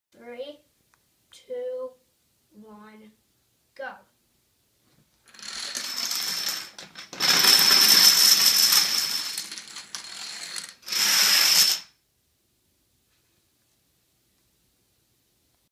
Domino run with a cascading wall
A few minor elements in this run including a nice cascading wall that I'm working on perfecting.